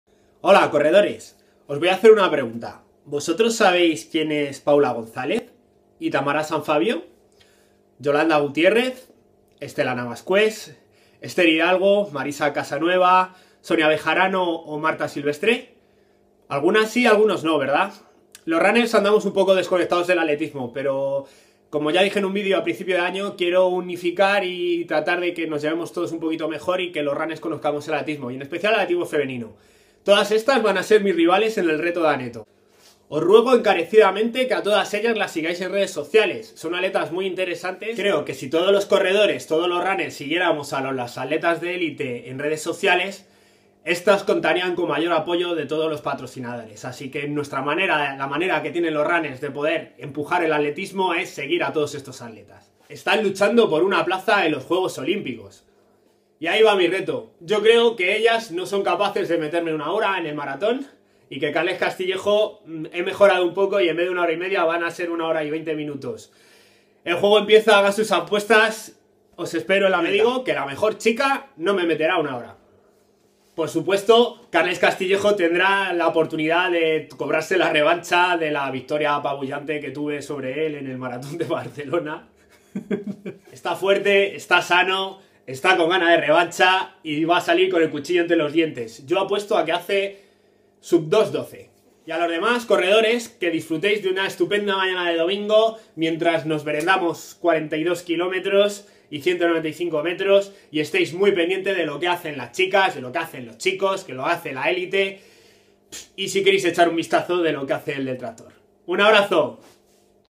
Maratón de Sevilla 2016 reto solidario KMXALIMENTOS con la élite femenina y Carles Castillejo.
Maratón de Sevilla 2016 reto solidario KMXALIMENTOS con la élite femenina y Carles Castillejo. Caldo Aneto